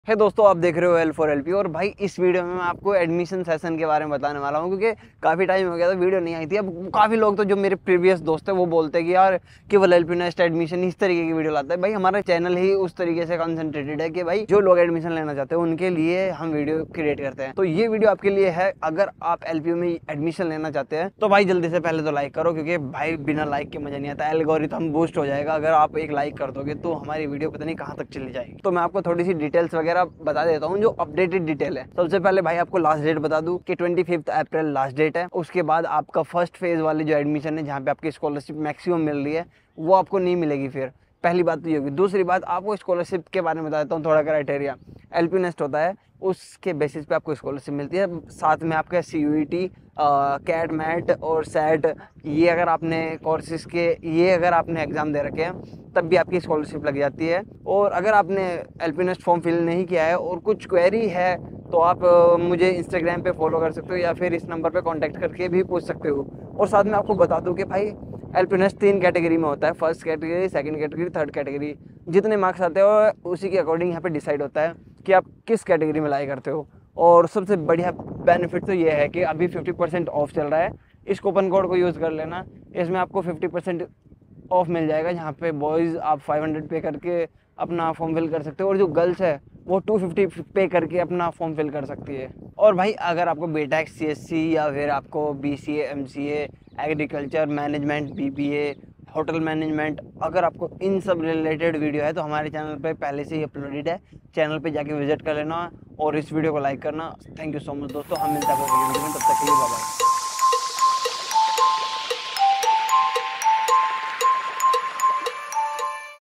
What is LPUNEST | LPU Scholarship | Last Date
LPUNEST, the Lovely Professional University National Entrance and Scholarship Test, is a key exam for admission and scholarships at LPU. It assesses candidates for various undergraduate and postgraduate programs through MCQs in subjects like Physics, Chemistry, and English. Flexible exam dates are available, but applicants must register online before the specified deadline. Missing the deadline may lead to forfeiture of the opportunity.